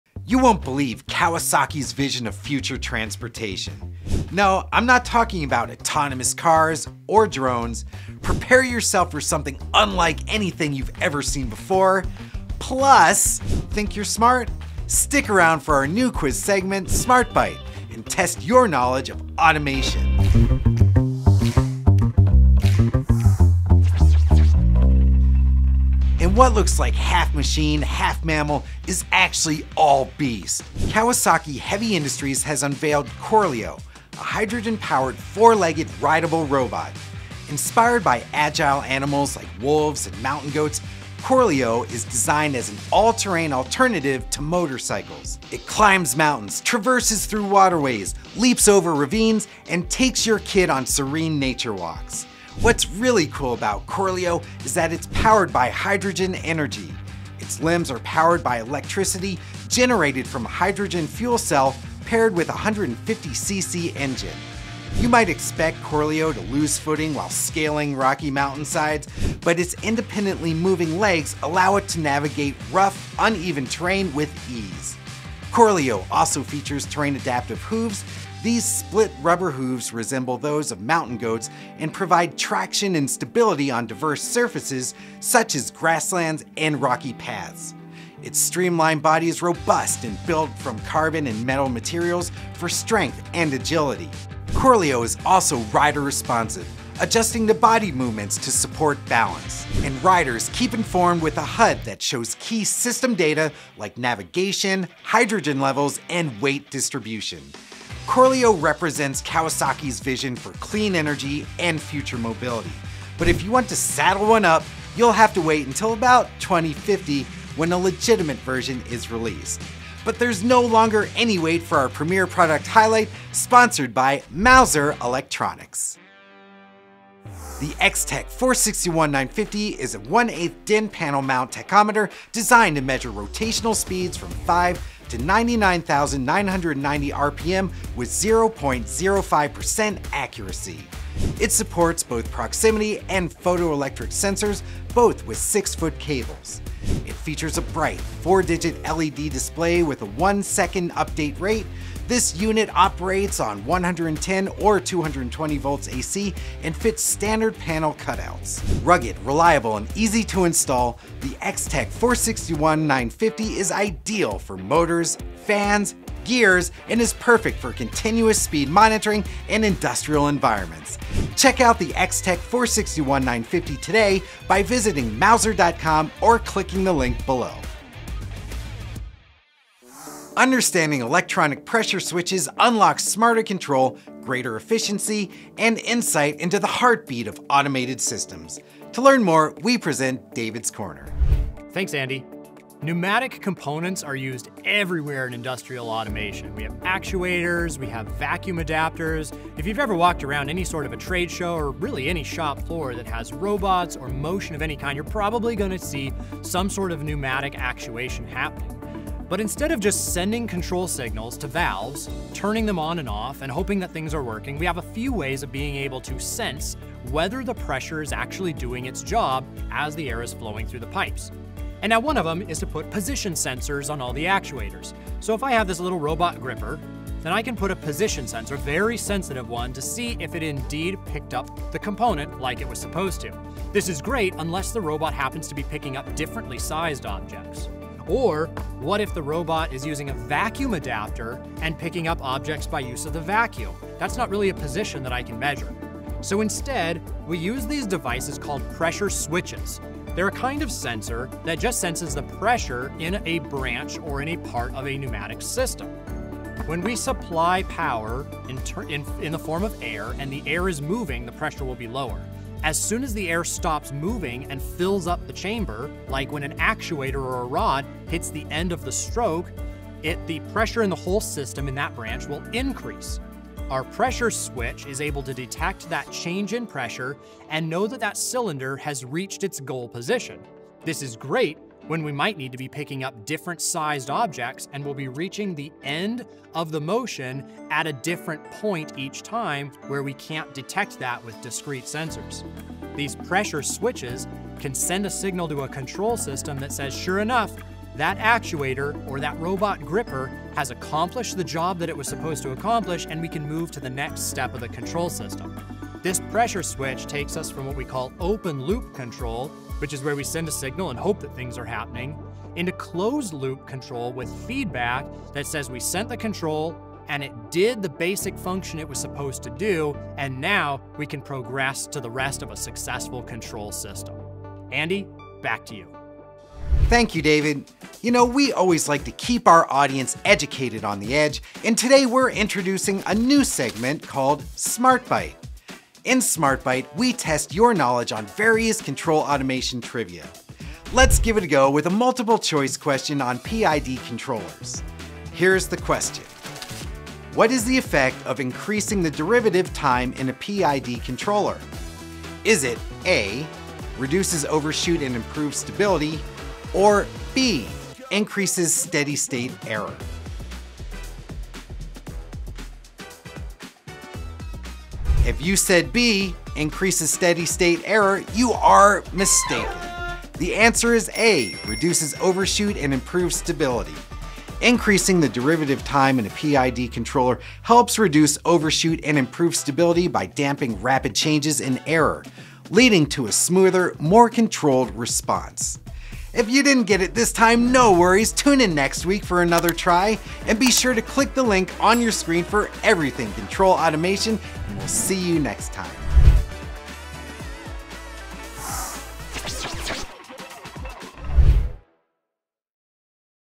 Automator's Edge | Ep. 54 | Corleo Scales the Future, Electronic Pressure Switches, Pop Quiz
In this edition of Automator’s Edge, the Control Automation team dives into Kawasaki's Corleo - an unbelievable vision of future mobility. Our premiere product highlight features the  Extech 461950, and control engineer, David Peterson, describes the function of a pressure switch, a particular kind of sensor that generates a signal output when pressure rises or falls beyond a set limit, in David's Corner. Also, think you're smart? Test yourself with our control automation quiz at the end and find out!

0:00 Intro
0:20 Kawasaki's Corleo is a futuristic beast!
2:06 Premiere Product Highlight
2:59 David's Corner: Electronic Pressure Switches
5:26 Pop Quiz yourself

Check out Control.com to explore news, technical content, and forum discussions to advance your journey in the world of control system engineering!